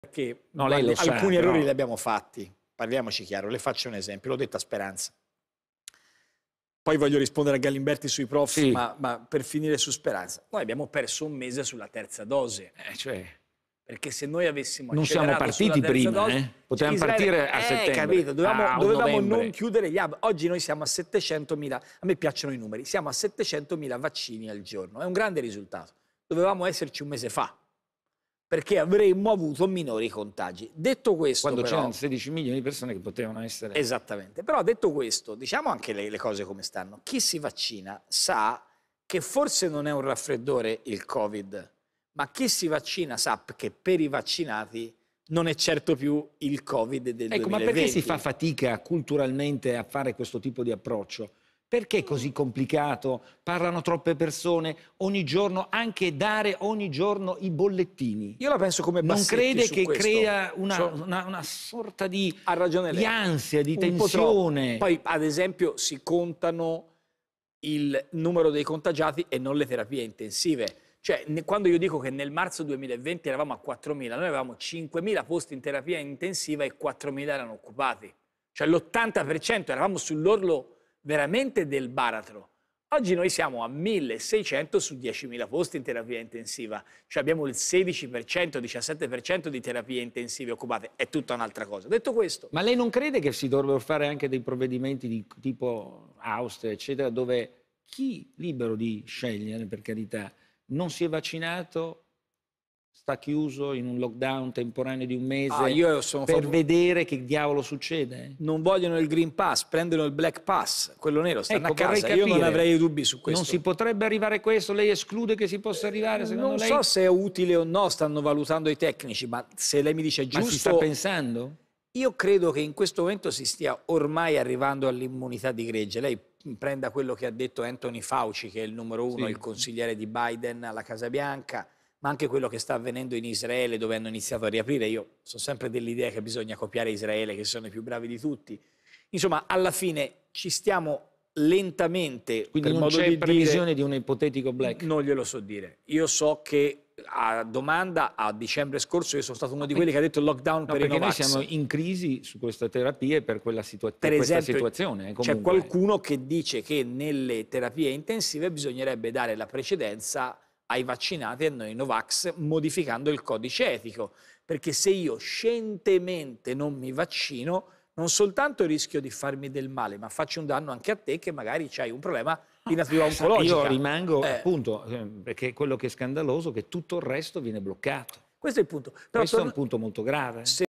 Renzi contro i no vax: "Io non ho dubbi, non vuoi il green pass prendi il black pass, stai a casa!"
Matteo Renzi intervistato da Massimo Giletti sui numeri dei contagi di Covid e la gestione della comunicazione.